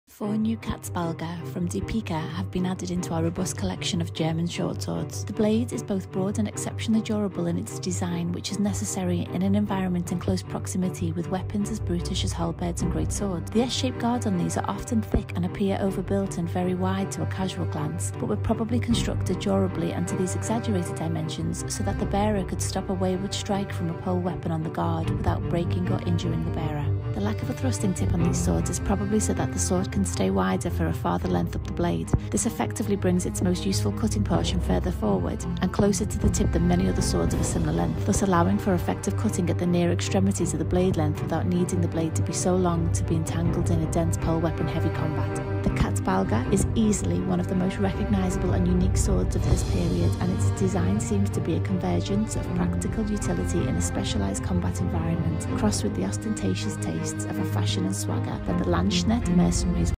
Range of Katzbalger Shortswords Unveiled | Kult of Athena
The famous German shortsword is updated in three new models at Kult of Athena.